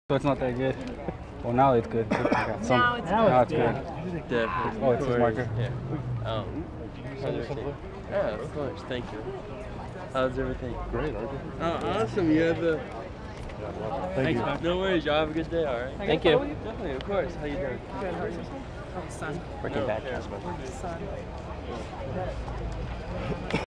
RJ Mitte greets fans at the 2013 Saturn Awards in Burbank...
RJ Mitte greets fans at the 2013 Saturn Awards in Burbank, 06/26/13 RJ Mitte greets fans at the 2013 Saturn Awards in  on June 26, 2013 in Los Angeles, California